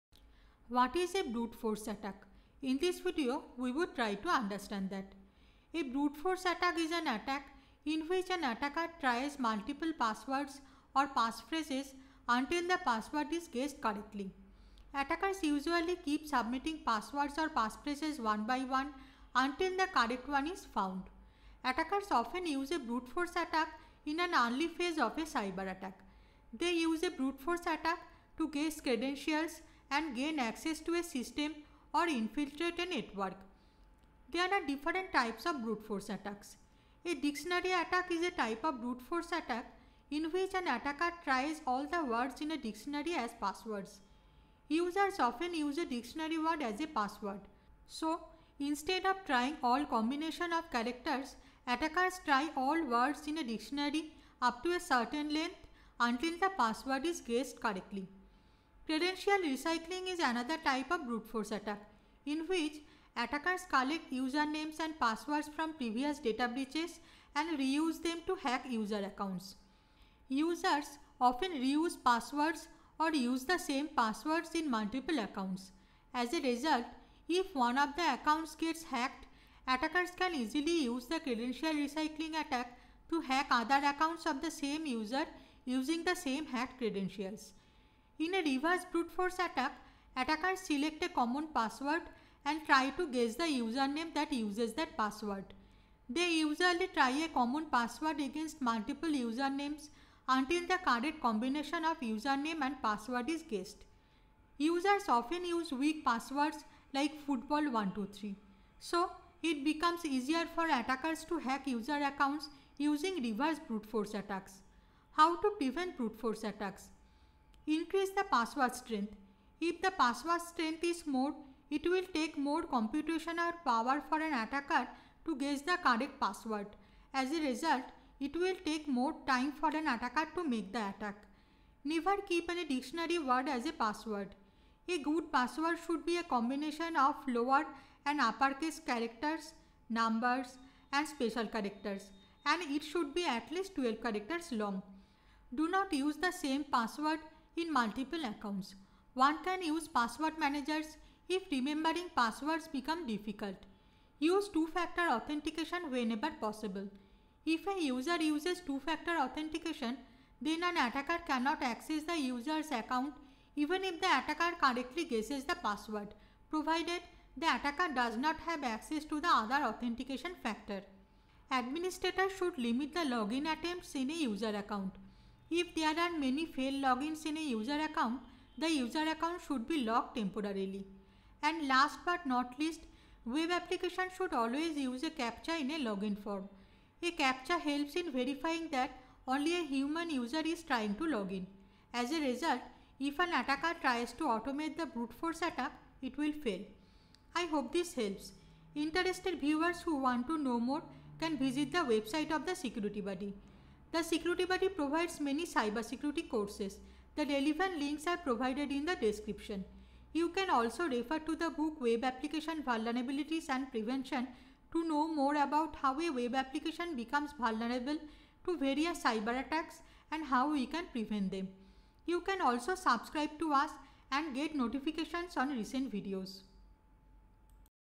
What is a brute-force attack?
This video explains what a brute-force attack is. Interested viewers may find the following links useful: